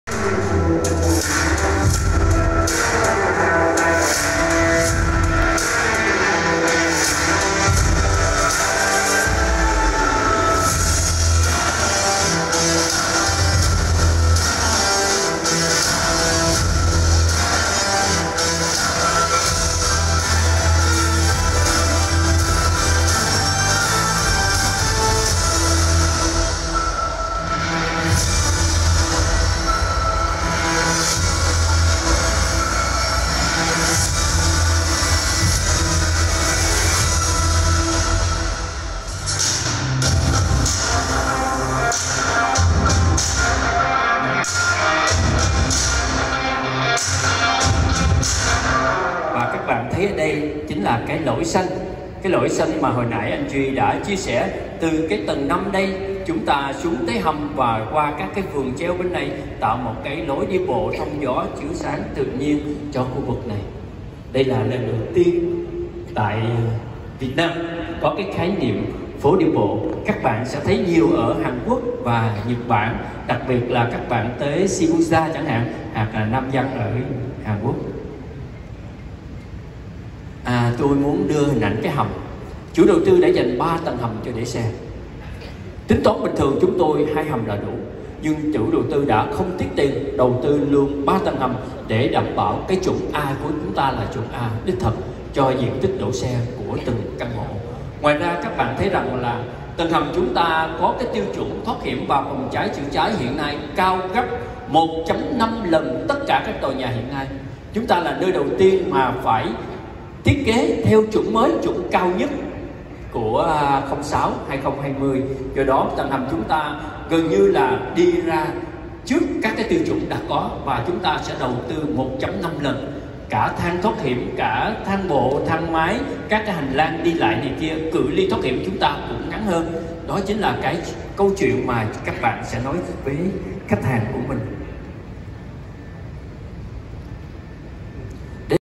Giới thiệu King Crown Infinity tại lễ ra quân dự án
Giới thiệu khu phức hợp thương mại và căn hộ cao cấp King Crown Infinity trung tâm Thủ Đức tại lễ ra quân dự án cùng 1.000 chuyên viên tư vấn từ các đại lý chính thức...